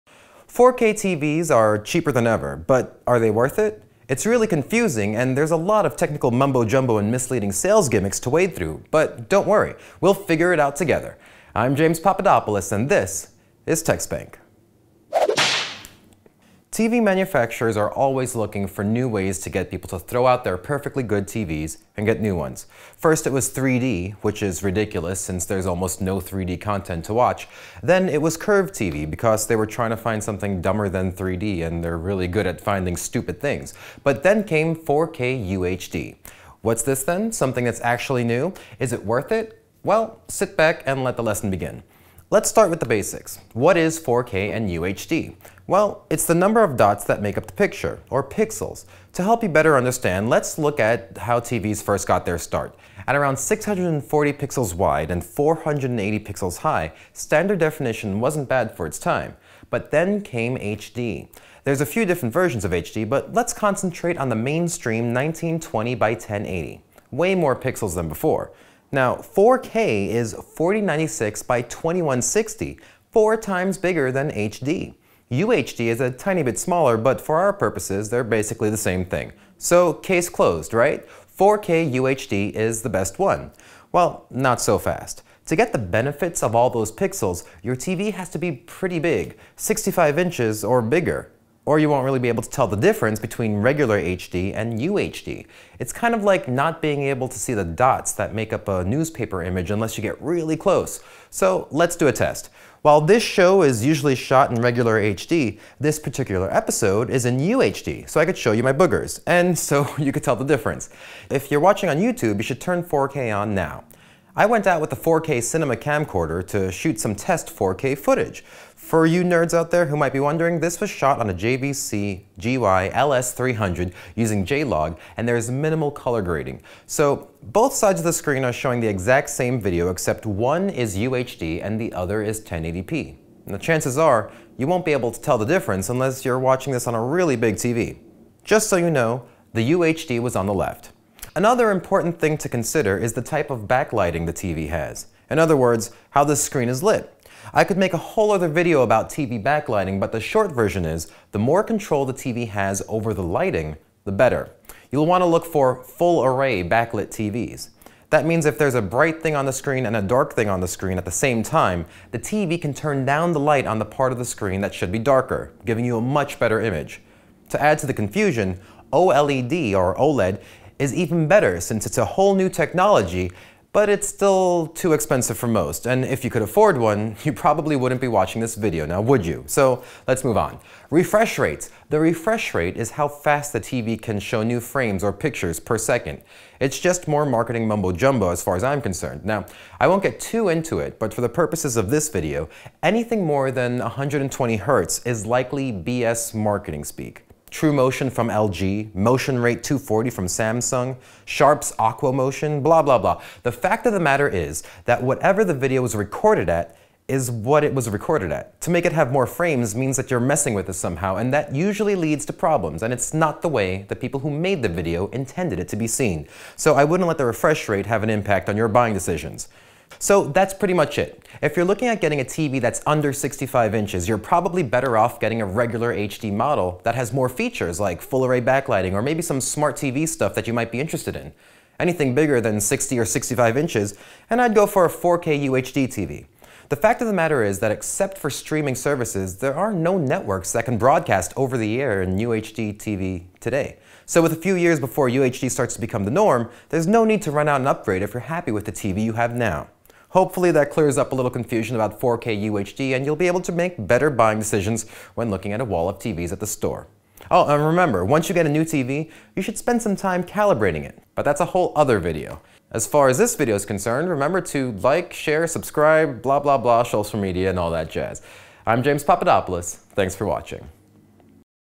Is 4K worth it?
4k and UHD TV’s are cheaper than ever, but are they worth the cost to upgrade?  This episode is available in 4k, but requires a computer/ device capable of 4K YouTube playback.

Transcript below:
It’s really confusing and there’s a lot of technical mumbo jumbo and misleading sales gimmicks to wade through, but don’t worry, we’ll figure it out together. I’m James Papadopoulos and this is techspank.

Now, I normally wouldn’t do an episode like this - not only is it super technical and confusing, but TV prices change all the time, people have personal preferences, and some people find some things more importent than others so in the next 5 minutes or so hopefully I’ll give you enough information to figure out what’s best for you.

TV manufacturers are always looking for new ways to get people to throw out their perfectly good TV’s and get new ones. First it was 3D - which is ridiculous since there’s almost no 3D content to watch. Then it was curved TV, because they were trying to find something dumber than 3d and they’re good at finding stupid things. But then came 4K UHD. What’s this then? Something that’s actually new? Is it worth it? Well, sit back and let the lesson begin.

Let’s start with the basics. What is 4K and UHD? Well… it’s the number of dots that make up the picture, or pixels. To help you understand, let’s look at how TV’s first started. At around 640 pixels wide and 480 pixels high, standard definition wasn’t bad for it’s time, but then HD came around. There’s a few different versions of HD but let’s concentrate on the mainstream 1920 by 1080… way more pixels than before. Now 4K is 4096 by 2160, 4 times bigger than HD. UHD is a tiny bit smaller, but for our purposes they’re basically the same thing.

So case closed, right? 4K UHD is the best one. Well… not so fast. To get the benefits that all those pixels bring, your TV has to be pretty big, 65 inches or bigger, or you won’t really be able to tell the difference between regular HD and UHD. It’s kinda like not being able to see the dots that make up a newspaper image unless you get really close. So let’s do a test. While this show is shot in regular HD, this particular episode is in UHD so I could show you this part. You should turn on 4K on YouTube now. I went out with a 4K cinema camcorder to shoot some test 4K footage - for you nerds who may be wondering, this was shot on a an overcast day using a JVC GY-LS300 using J-Log and there’s minimal color grading. So both sides are showing the exact same video, except one is UHD and the other is 1080p. Chances are you won’t be able to tell the difference unless you’re watching this on a really big TV. Just so you know, the UHD was on the left.

Another important thing to consider is the type of backlighting the TV has - in other words, how the screen is lit. I could make ANOTHER whole video about TV backlighting, but the short version is, the more control the TV has over lighting, the better. You’ll want to look for full array backlit TV’s. That means if there’s a bright thing and a dark thing on the screen at the same time, the TV can turn down the light on the part of the screen that should be darker, giving you a much better image. To add to the confusion, OLED is even better since it’s a whole new technology but it’s still too expensive for most and if you could afford one you wouldn’t be watching this video, would you, so let’s move on.

Refresh rates! The refresh rate is how fast the TV can show new frames or pictures per second. It’s just more marketing mumbo jumbo as far as I’m concerned. I won’t get TOO into it, but for the purposes of this video, anything more than 120hz is likely BS marketing speak. TruMotion from LG, Motion Rate 240 from Samsung, Sharps AquoMotion, blah blah blah. The fact of the matter is that whatever the video was recorded at, is what it was recorded at, to make it have more frames means that you’re messing with it somehow and that usually leads to problems and it’s not the way the people who made the video intended it to be seen, so I wouldn’t let the refresh rate have an impact on your buying decisions.

So that’s pretty much it - if you’re looking at getting a TV that’s under 65 inches, you’re probably better off getting a regular HD model that has more features like full array backlighting or maybe some smart TV stuff that you might be interested in. Anything bigger than 60 or 65 inches and I’d get a 4K UHD TV. The fact of the matter is that except for streaming services, there are no network that can broadcast over the air in UHD today. So with a few years before UHD starts to become the norm, there’s no need to upgrade if you’re happy with the TV you have now.

Hopefully that clears up a little confusion about 4K UHD, and you’ll be able to make better buying decisions when looking at a wall of TV’s at the store. Oh, and remember, once you get a new TV, you should spend some time calibrating it, but that’s a whole other thing...